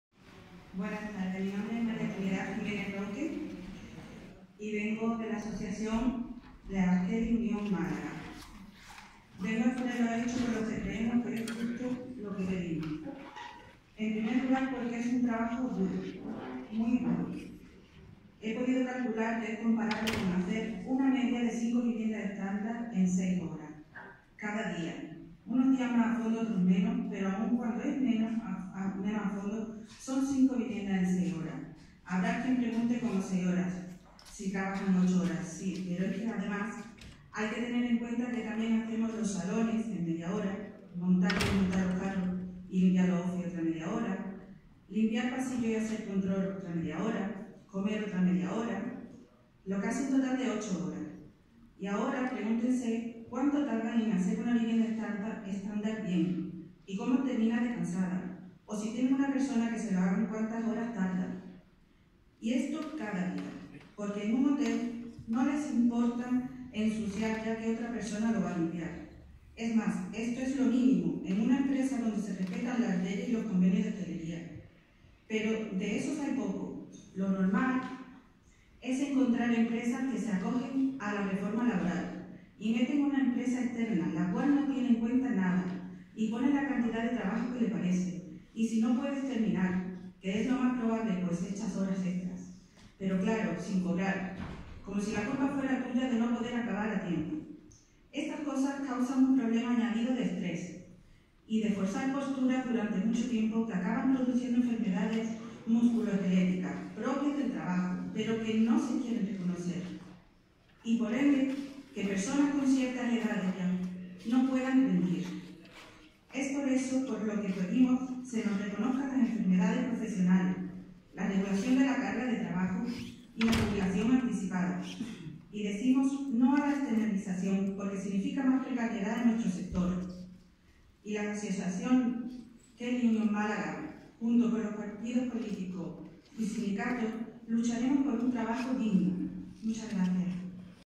Las Kellys hablan en la comisión de Economía
Intervencion de las kellys Union Málaga en la comisión de Economia, Hacienda, Recursos Humanos, Reactivación Económica, Promoción Empresarial, Fomento del Empleo y Turismo.